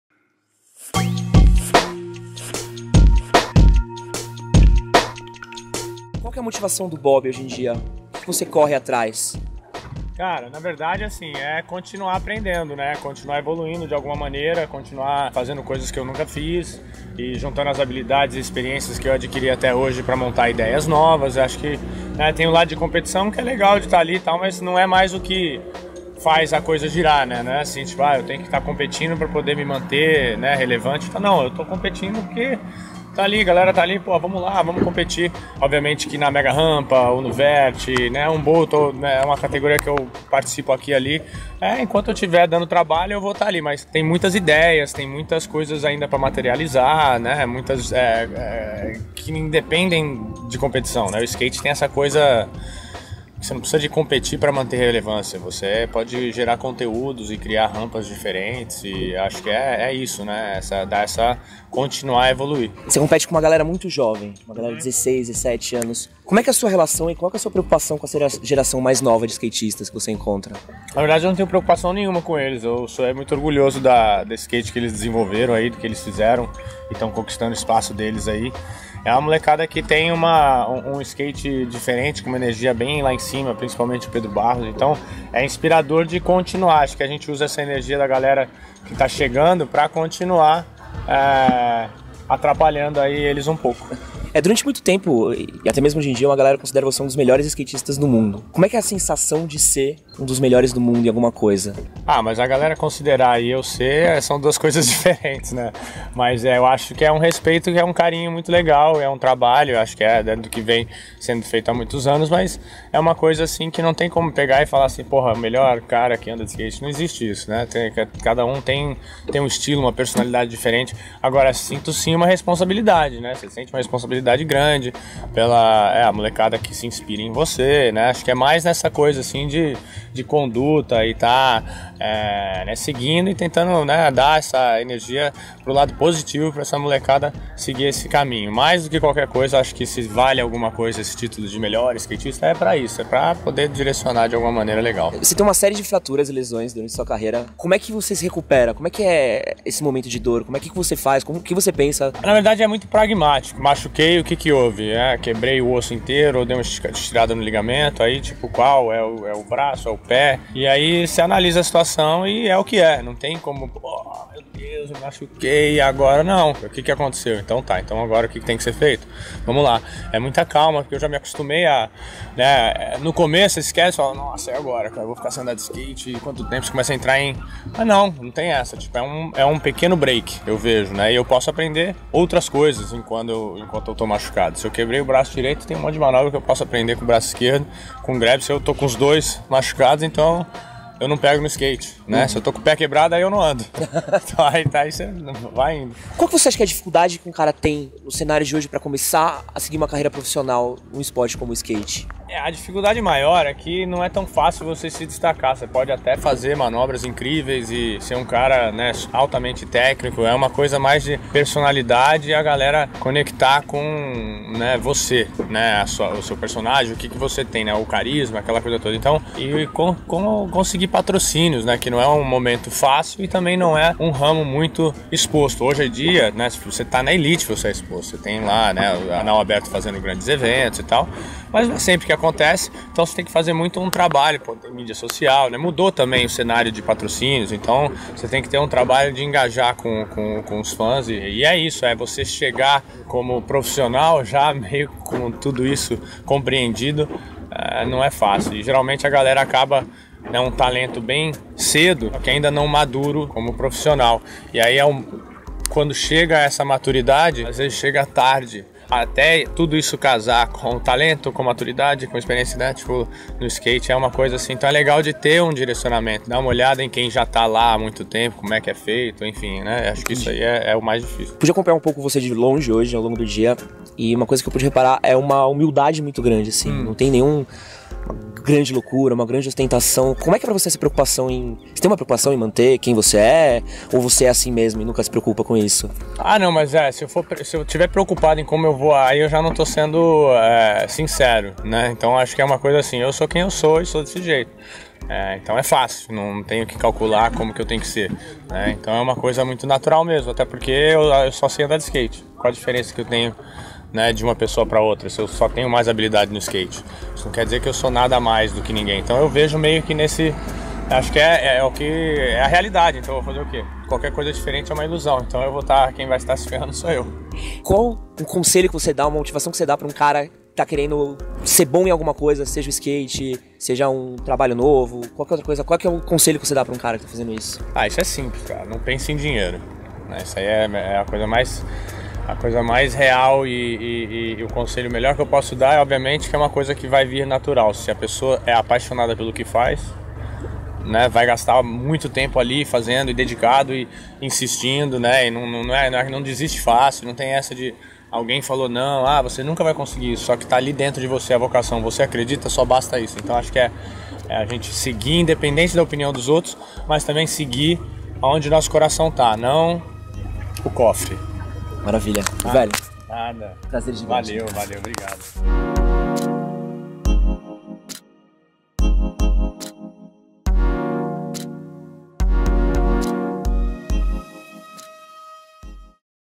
7 perguntas para Bob Burnquist
O MHM foi até o Rio de Janeiro para entrevistar Bob Burnquist, um dos maiores skatistas do mundo e vencedor de 28 medalhas no X-Games.

Nascido no Rio de Janeiro, Bob Burnquist é recordista de medalhas na competição, já estrelou jogos de videogames e é patrocinado pelas mais diversas marcas. Mas como será que o cara é no mano a mano?